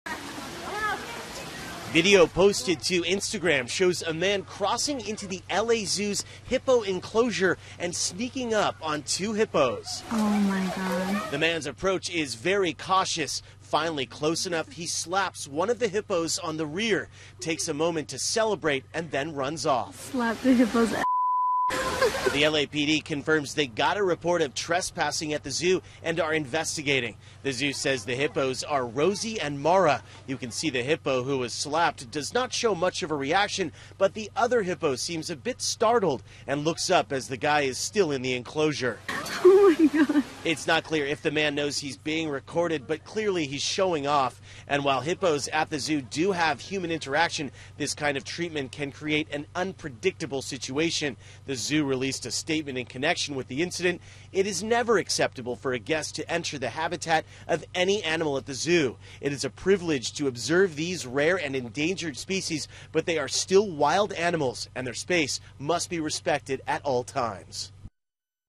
Police look into video of man spanking hippo at LA Zoo
Police are investigating after a video shows a man spanking a hippopotamus at the Los Angeles Zoo.